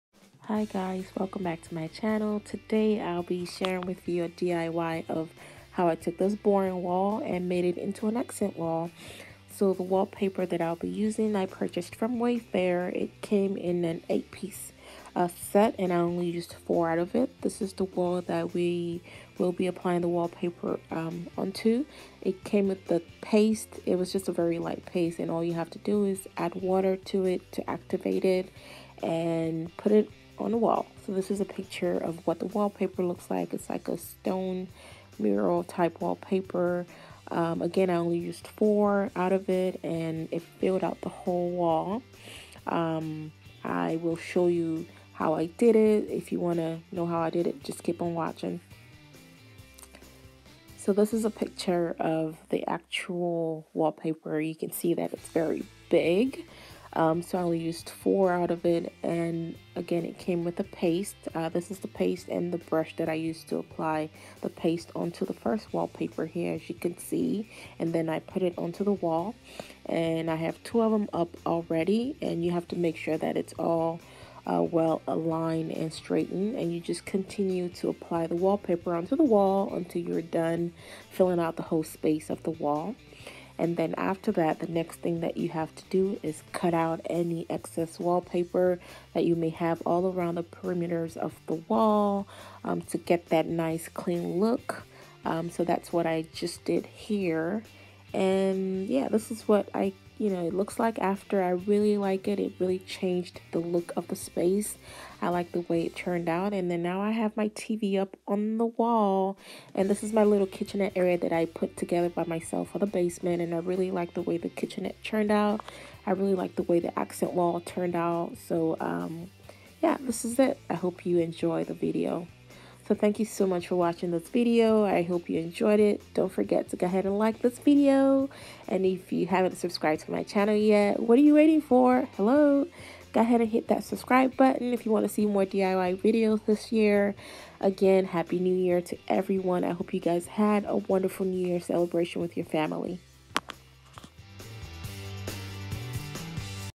DIY WALLPAPER (STONE WALL MURAL)
Hi guys!!

This video is a diy wallpaper application for an accent wall! If you have a wall in a room that is just plain and boring, spice it up with wallpaper as such to add interest to the room.